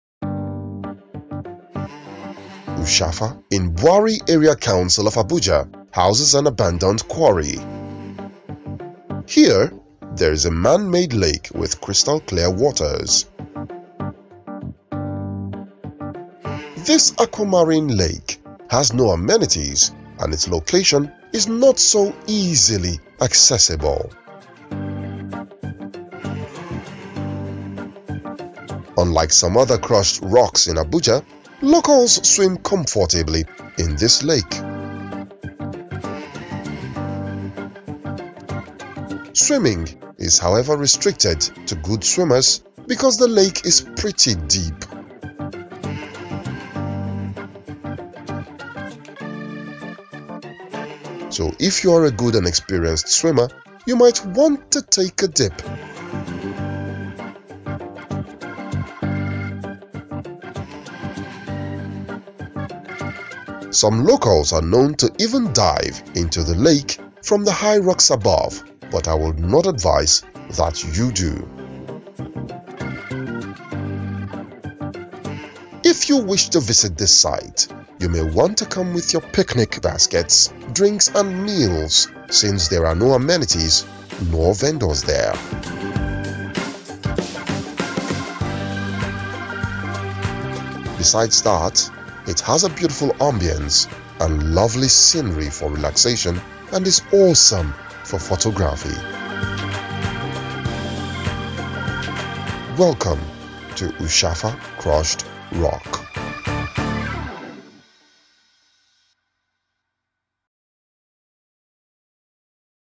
USHAFA CRUSHED ROCK (FROM THE EYES OF THE gODS
One exotic natural location you should visit when in Abuja in Nigeria is USHAFA CRUSHED ROCK. There are some things you need to note before you visit,

Overall, it makes for an amazing experience!!!